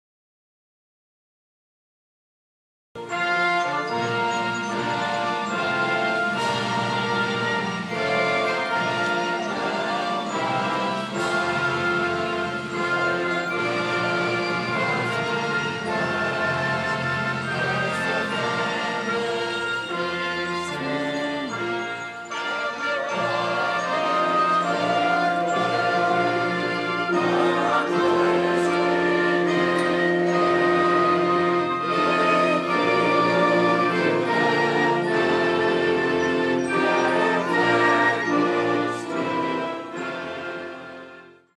Memorial Day
Memorial Day 2013 at Rancho San Luis Rey in Oceanside, CA.  A very patriotic and moving flag ceremony.